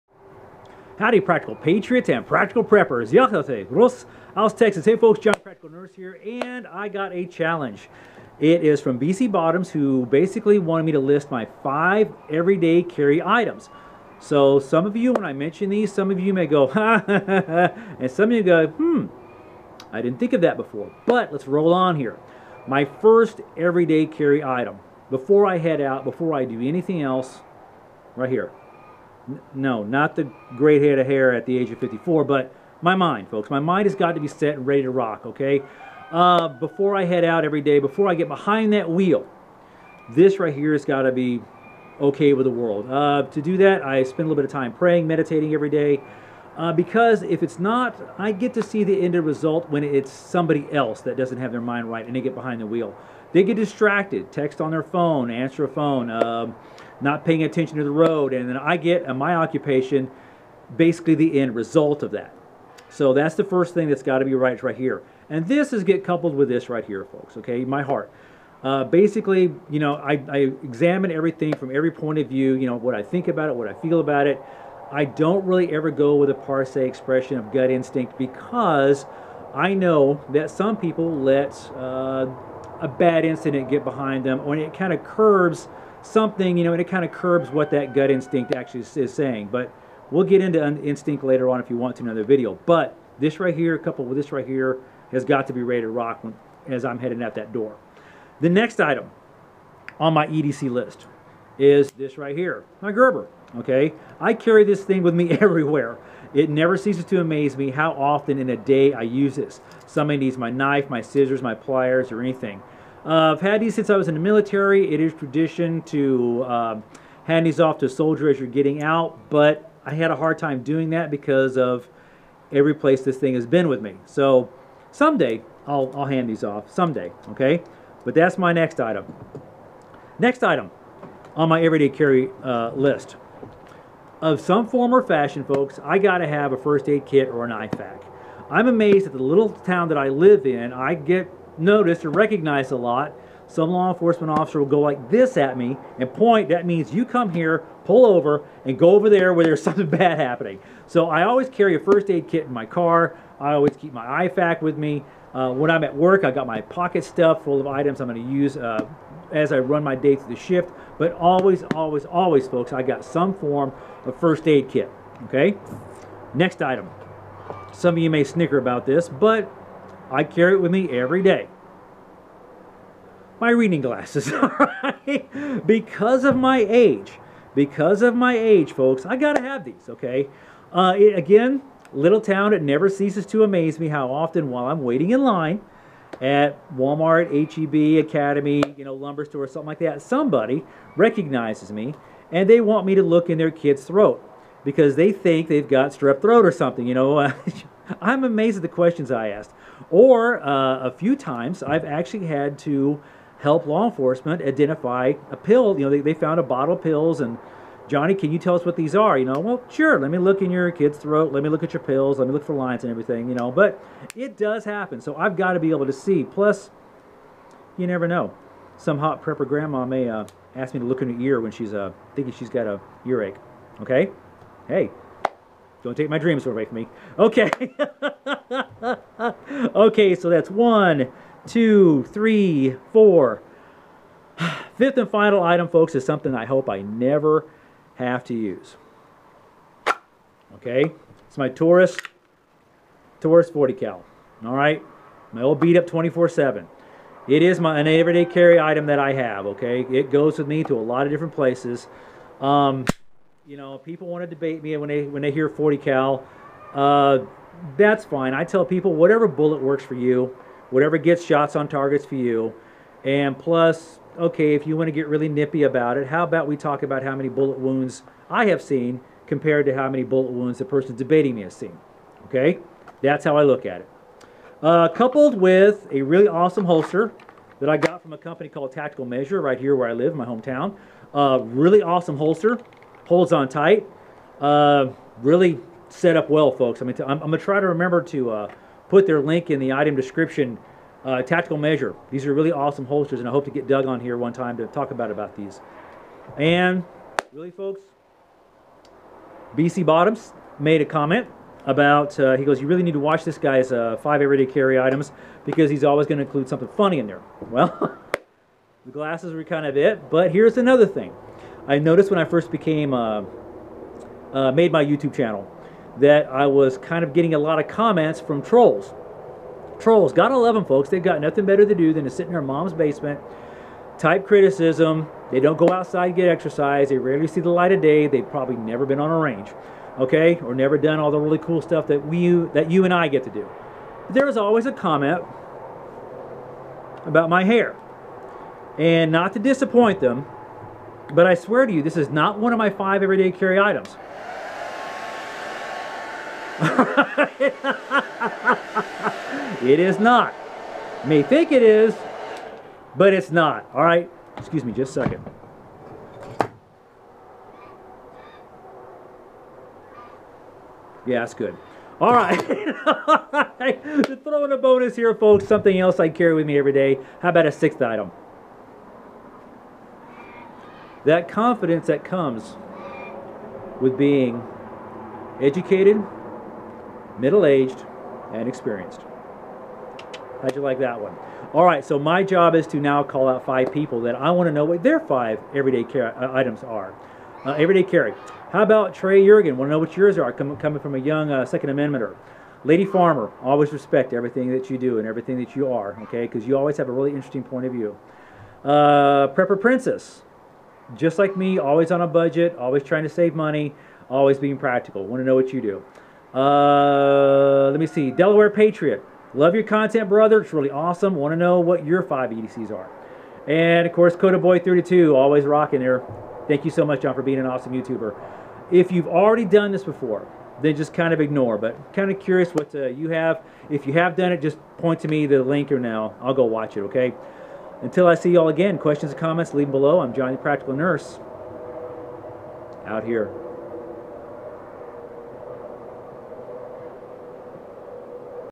My 5 EDC Challenge!!
BC Bottoms challenged me to show my 5 EDC items, now I pass that challenge off to Trey Yergen, Lady Farmer, Prepper Princess, Delaware Patriot, and Kotaboy32 Tactical.